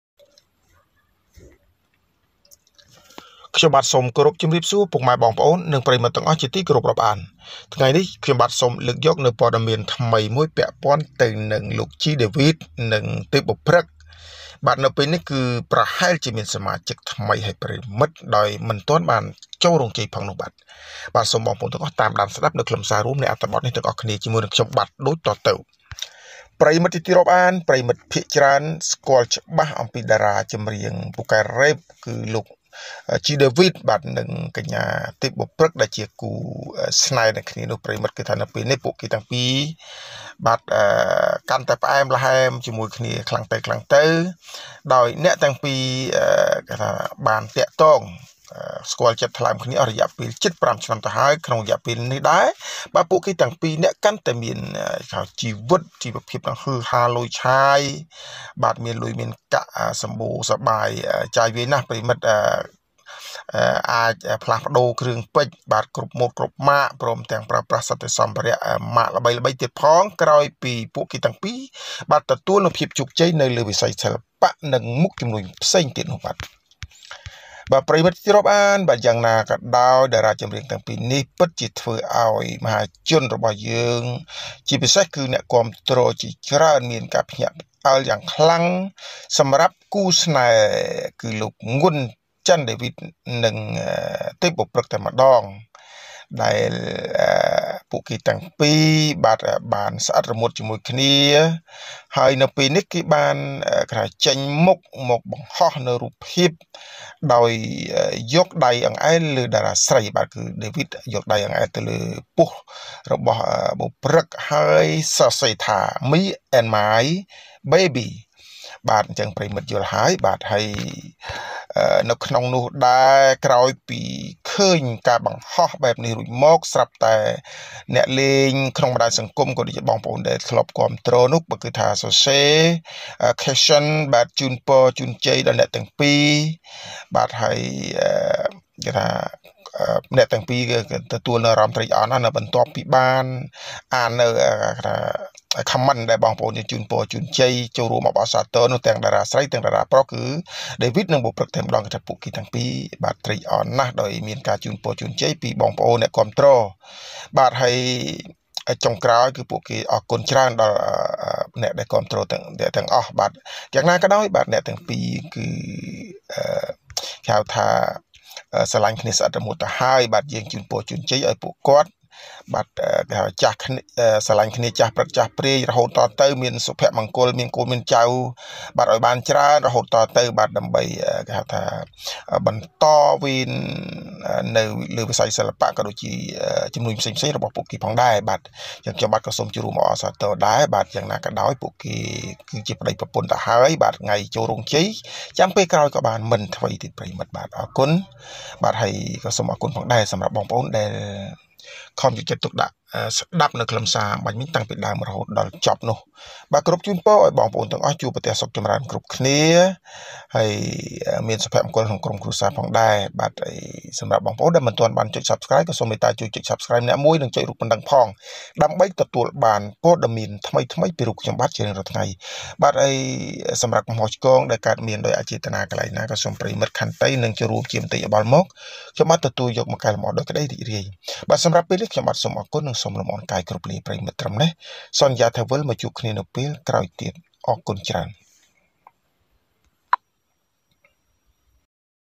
Congratulations! G. David and Tep Boprek may have new members
អបអរសាទរ! លោក ជី ដេវីត និងទេពបូព្រឹក្ស ពេនេះប្រហែលជាមានសមាជិកថ្មី